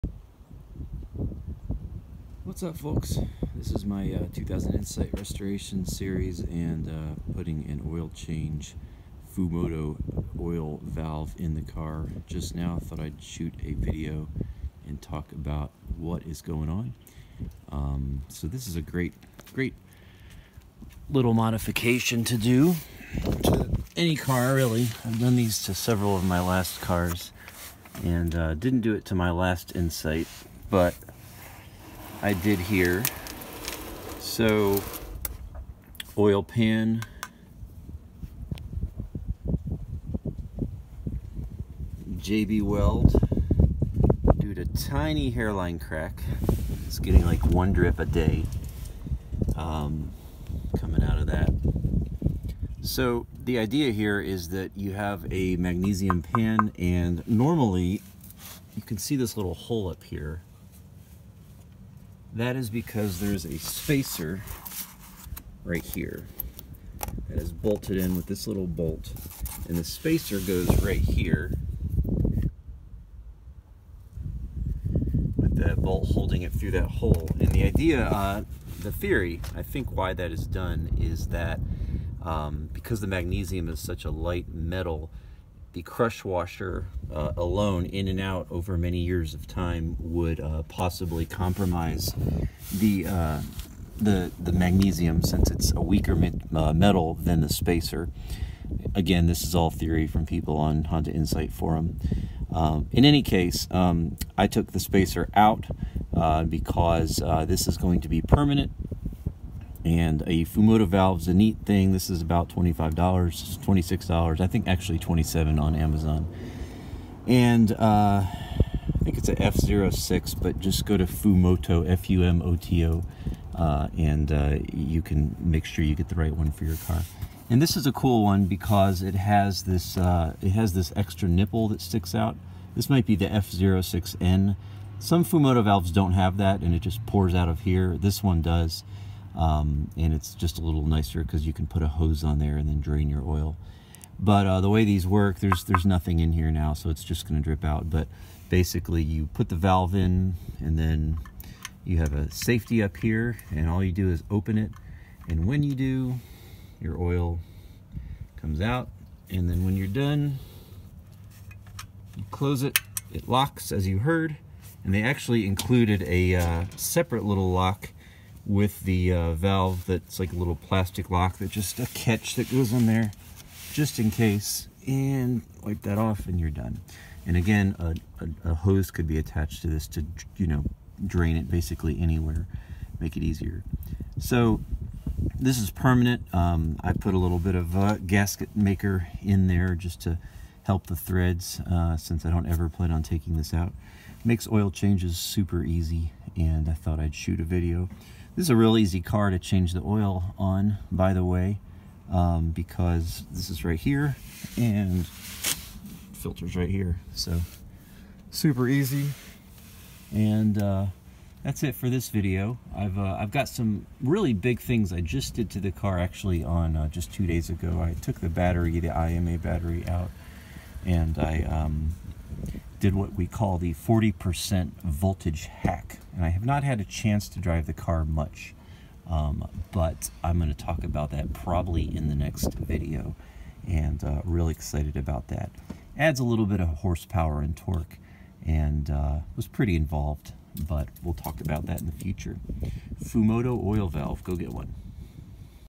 2000 Honda Insight restoration video part 7: Fumoto oil valve and RMM
Didn't mention the rear motor mount in the video but that just got installed also.  If you want more info on that job check my channel, I installed one on my blue Insight also and went through how to do it in a previous video.